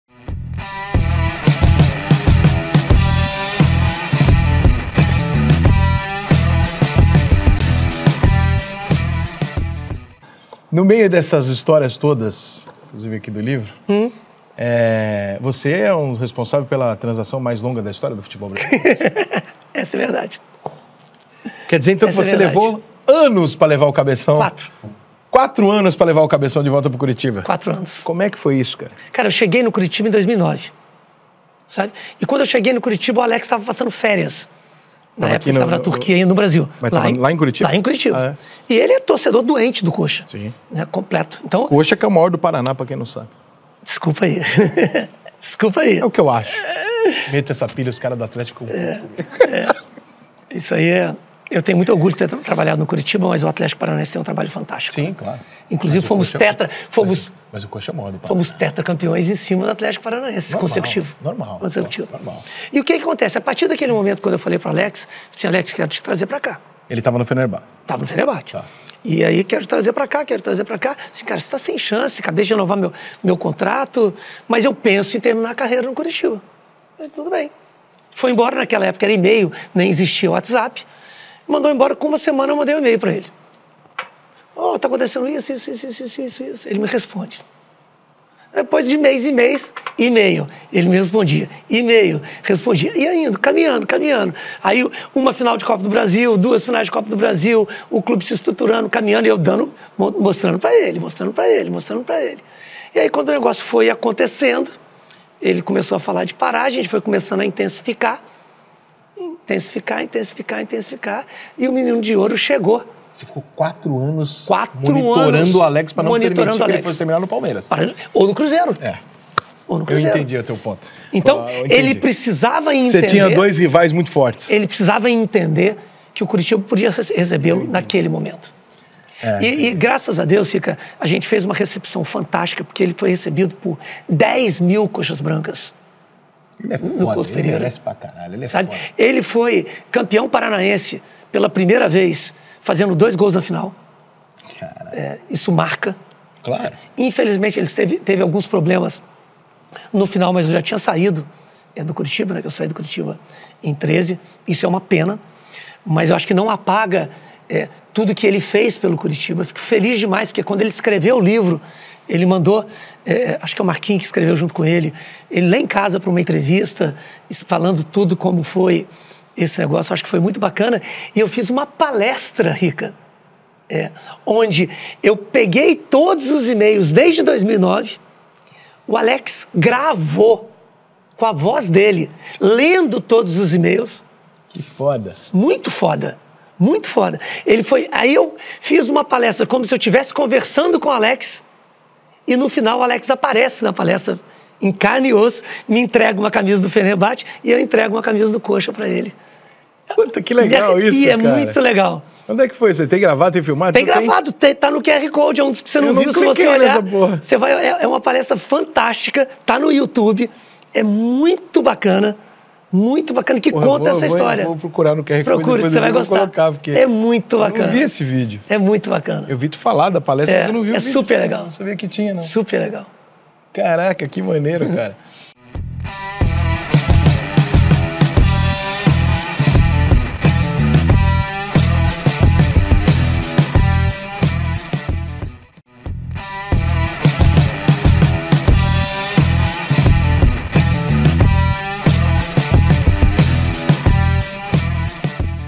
TRANSFERÊNCIA DE ALEX PARA O CORITIBA FOI A MAIS LONGA DA HISTÓRIA DO FUTEBOL
Felipe Ximenes foi  um dos principais responsáveis pela transferência de Alex para o Coritiba. Ele se recorda que a negociação com o meia levou quatro anos, pois sabia que teria a concorrência de Palmeiras e Cruzeiro

Politica de cortes: Ninguém está autorizado a usar cortes e trechos das entrevistas do Cara a Tapa em NENHUMA plataforma digital. Caso aconteça, iremos notificar.

Produção: Estúdio Century